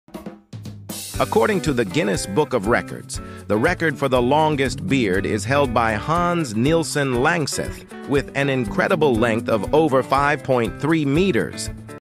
The longest beard in the world
🧔 Hans Nilsson Langseth became an icon with the longest beard in the world - an incredible 5.3 meters! 🌟 Dive with us into the world of records as we explore this natural wonder. 📏✨ Could you imagine a beard longer than the average car? Discover more in 20 seconds! 🎥 records longest beard